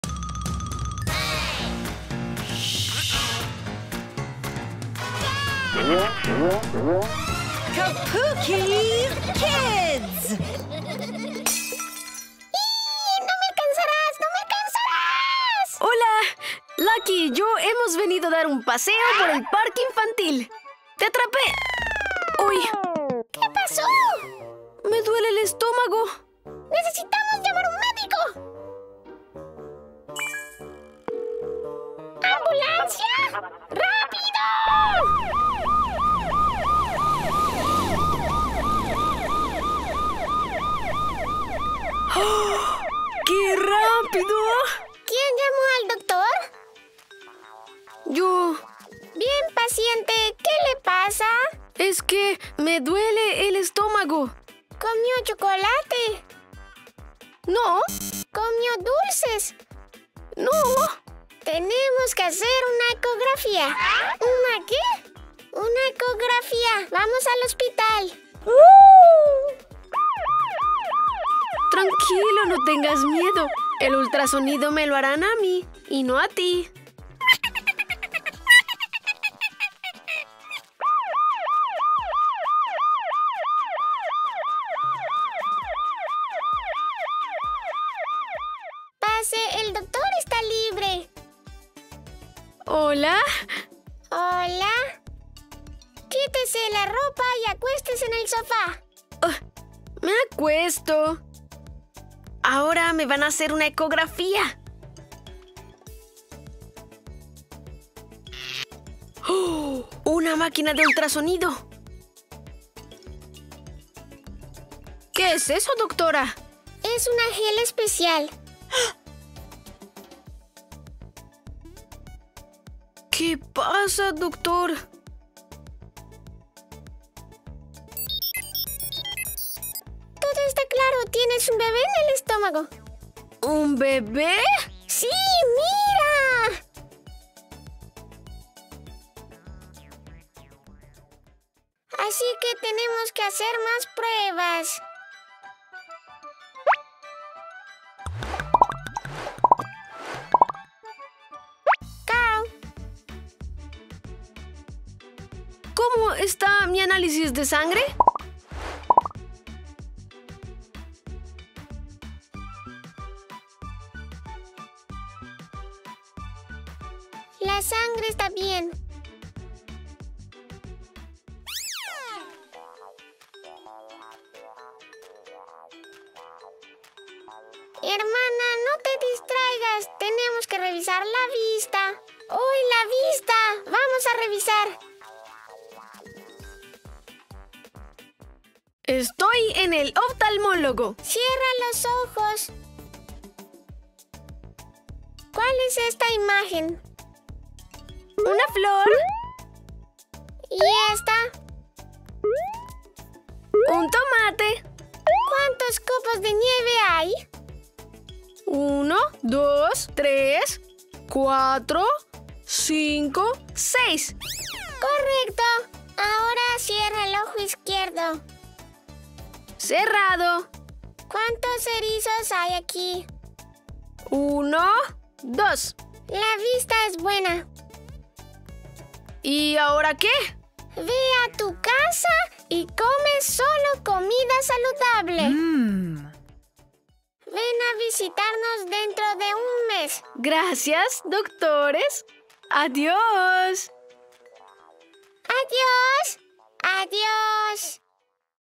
Mamá viene a ver a la doctora Bianca. Vídeos para bebés. Como mamá.
Vídeos para bebés en el canal “Como mamá”. ¡Mamá está jugando con Lucky cuando de repente le empieza a doler el estómago! ¡Necesitamos llamar a la doctora Bianca! Primero haremos una ecografía... ¡Vaya, parece que la madre tiene un bebé en el vientre! Ahora hay que revisar la vista de la mamá y darle recomendaciones sobre su dieta..